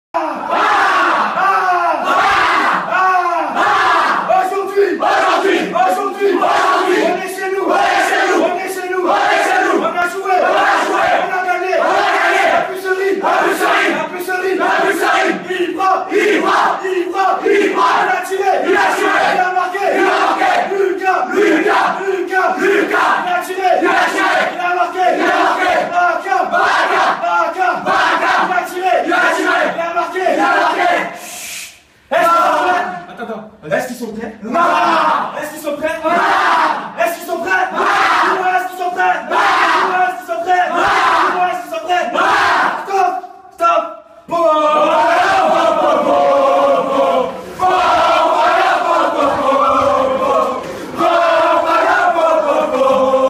Venelles fête la victoire
Victoire (3-2) de l'Us Venelles sur l'As Busserine en U17 Excellence.